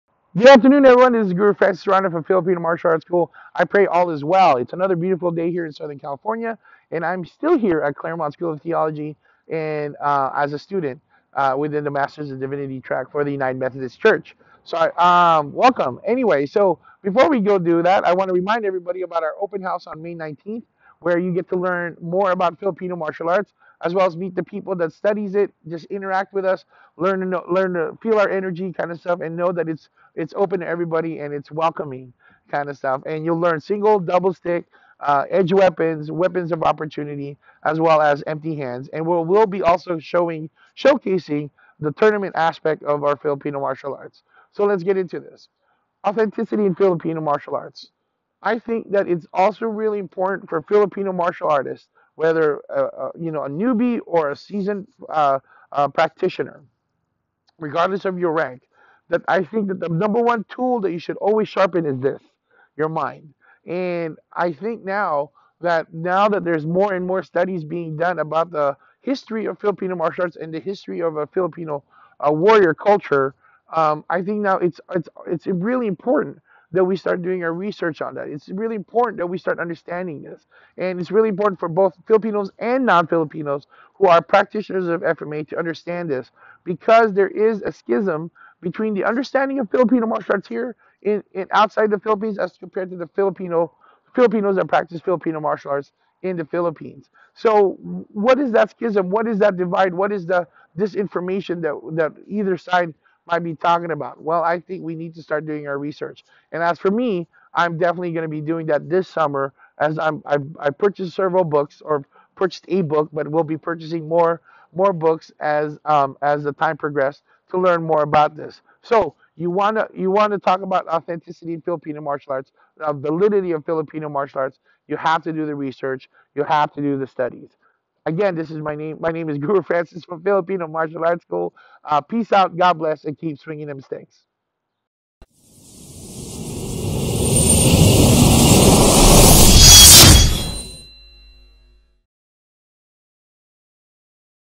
Sharpen the most lethal weapon... Your mind
The most lethal weapon in a person's arsenal is their mind. Don't just sharpen your physical skills but enhance your mental one as well.